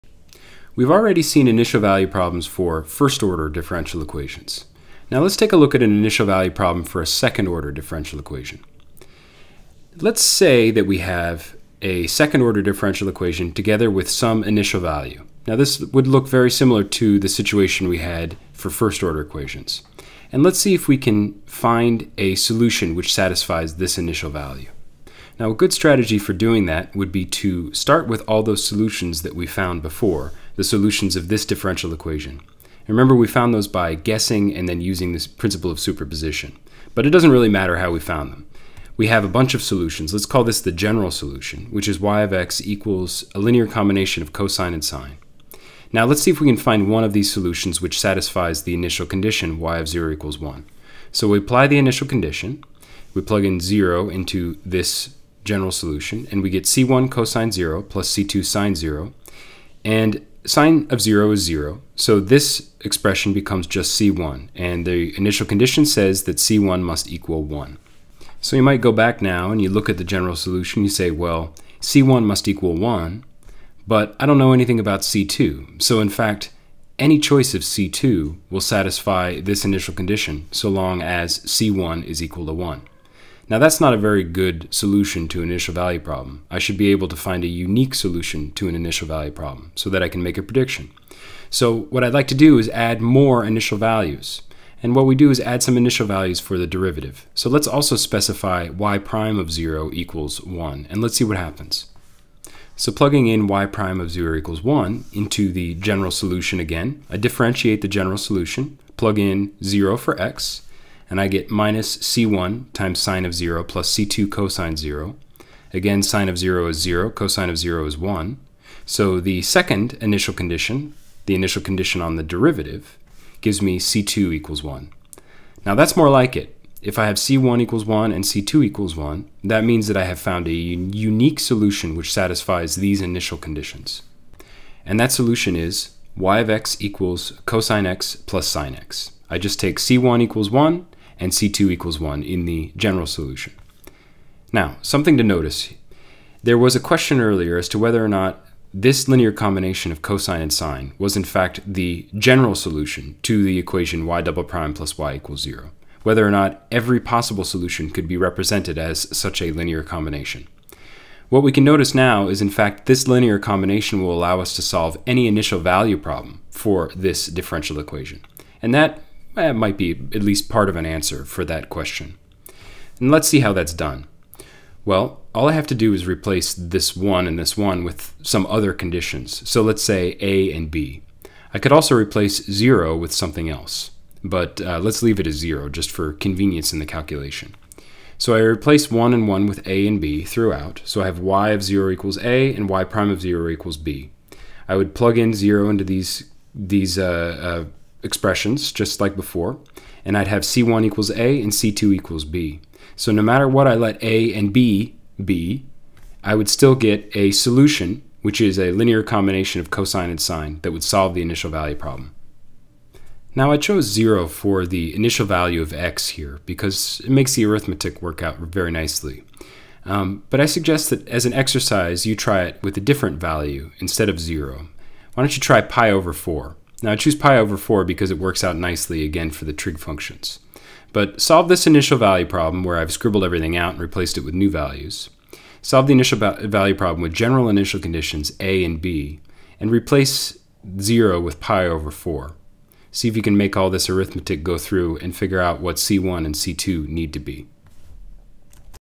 ODE | Initial value problems for second order equations
Examples and explanations for a course in ordinary differential equations.

We define and solve an initial value problem for a second order linear differential equation, using solutions found earlier.  We also discuss the idea of being able to solve *any* initial value problem.